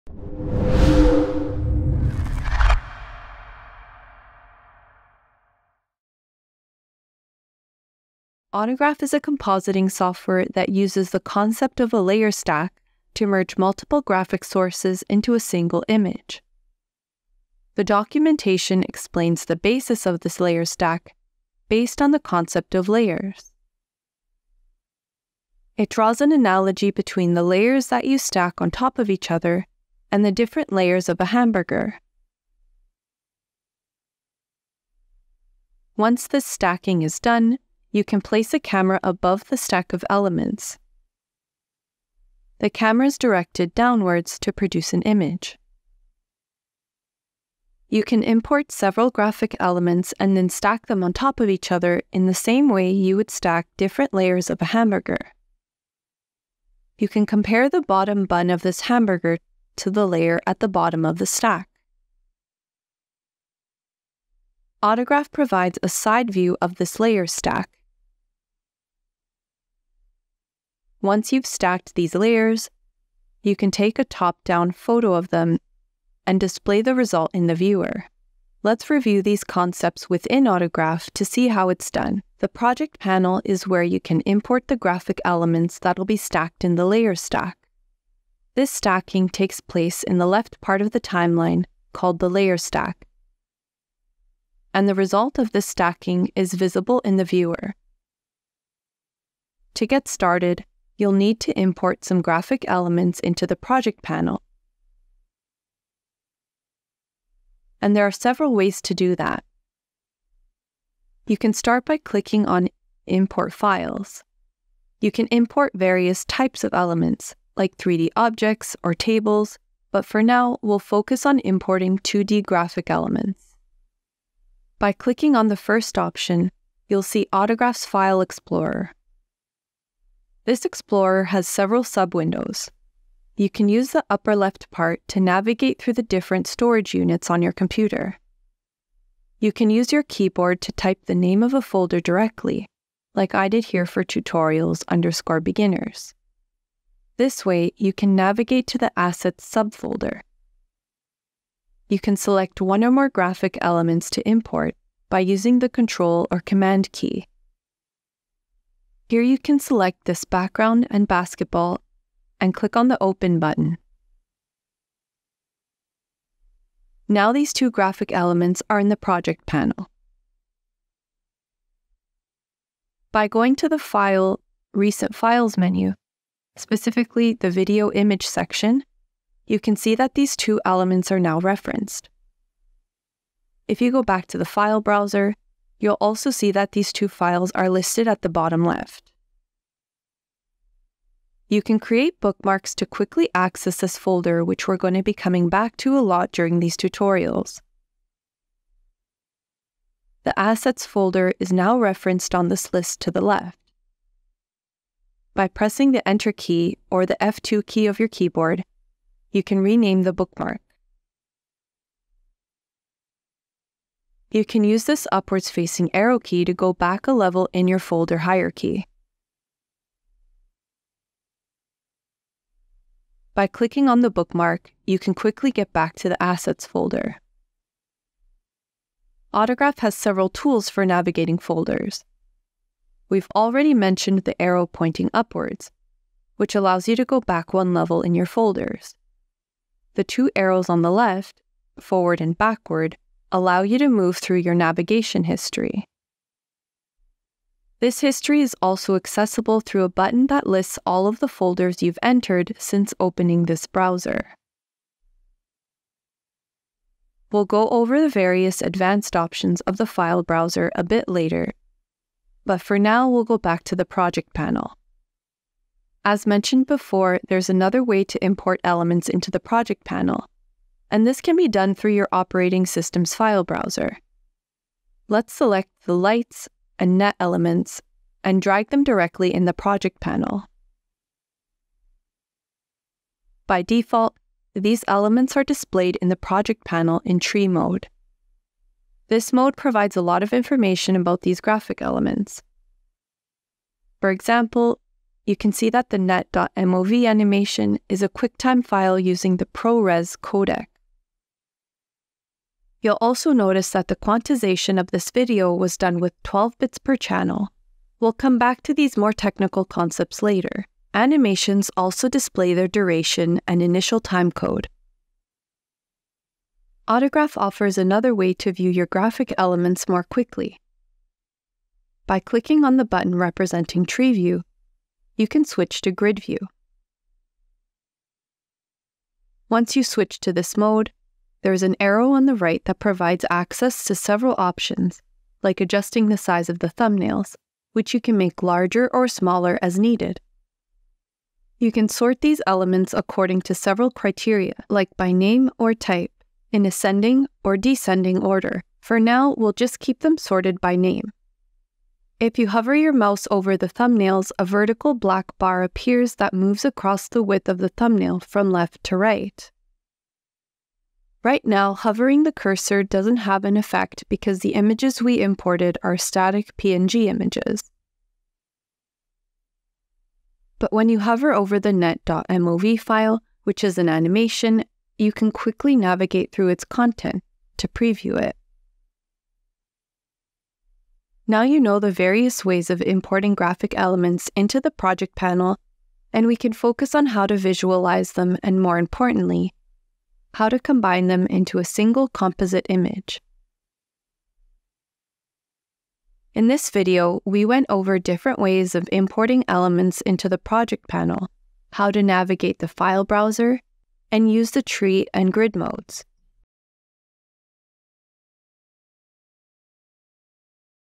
Importing Elements in the Project Panel and Using the File Browser
In this video, you will learn different ways of importing elements into the Project Panel, how to navigate in the File Browser, and how to use the Tree and Grid modes.

0:00 Intro
0:17 The concept of layers
01:18 General panel overview
01:39 Importing elements into the Project Panel using Autograph's File Browser
02:52 Accessing recent files
03:11 Creating Bookmarks
03:34 Autograph File Browser navigation tools
04:25 Importing elements into the Project Panel using your device file browser
04:43 Project Panel Tree Mode
05:16 Project Panel Grid Mode
05:52 Project Panel quick thumbnail preview for videos/animated footage
06:21 Conclusion

This video has subtitles for the hearing impaired and to help with automatic subtitle translation. While automatic translation will not be perfect, you can set the subtitles to your preferred language to better follow along.